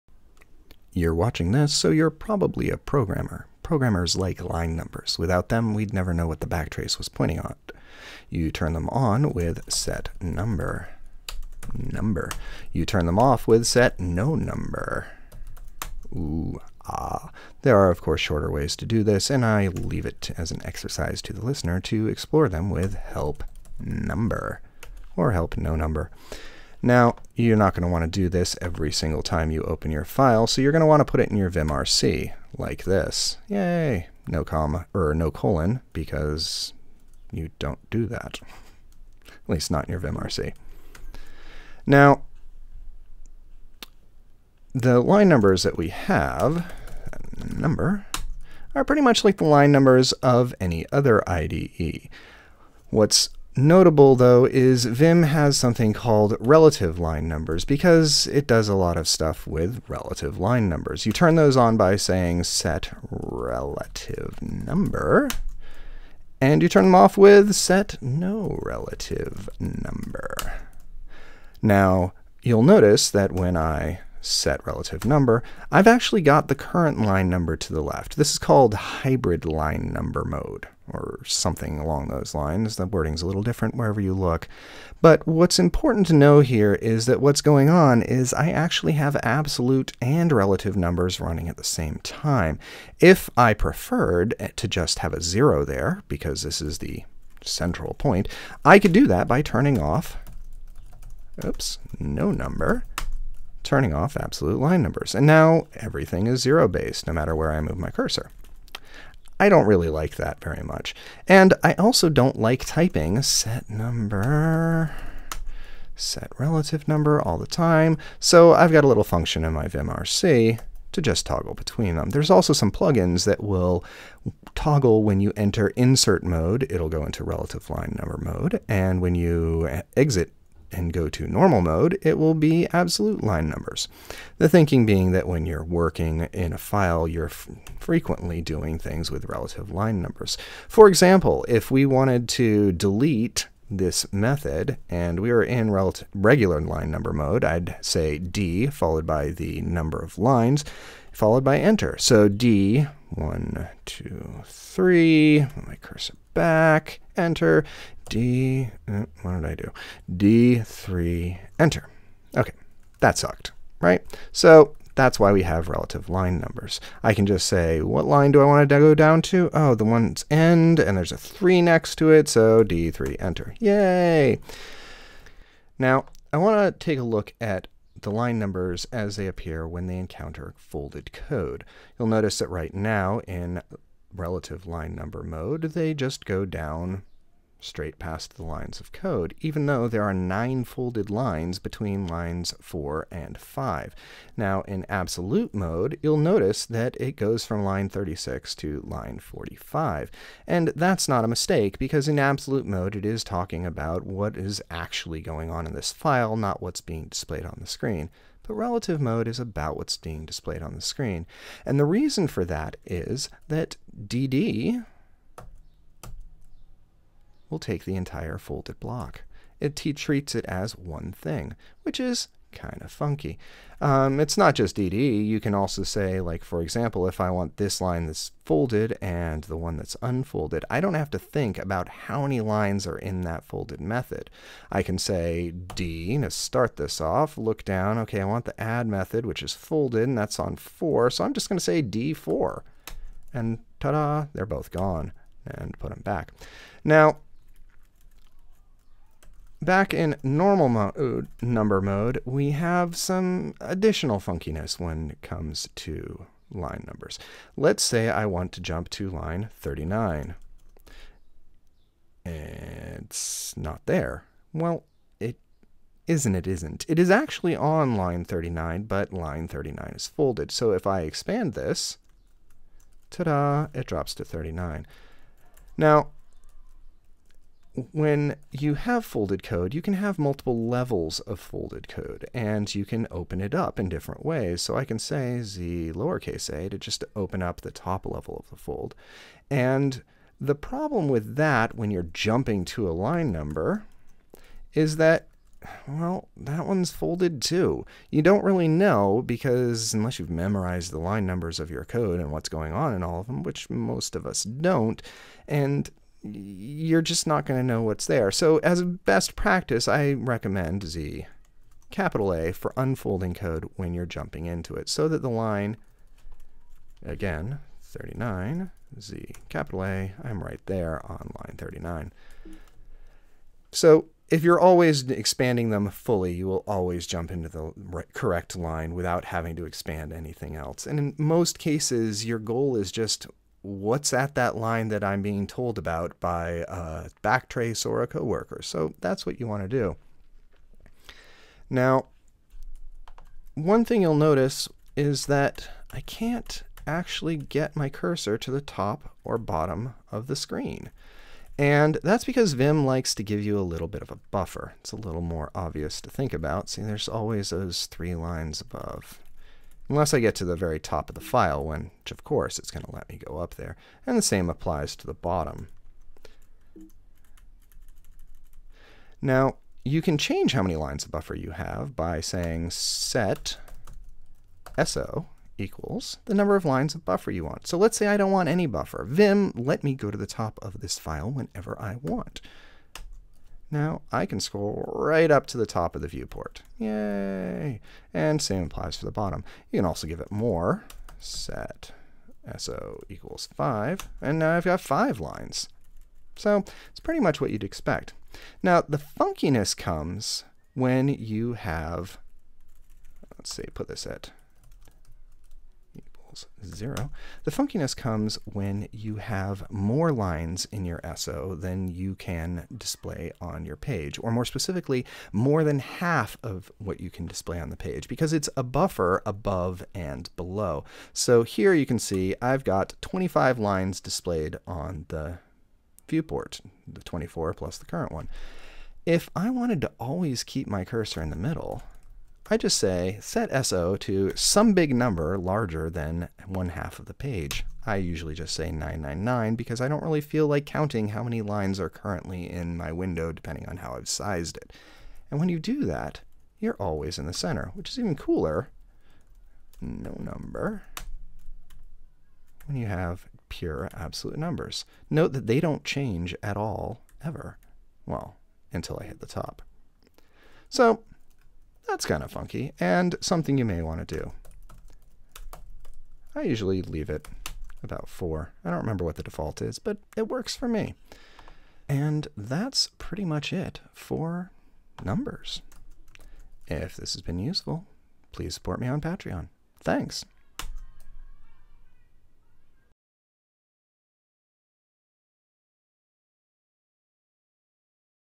Line Numbers in Vim
Absolute, relative, hybrid, toggling. How they interact with folded code, and more.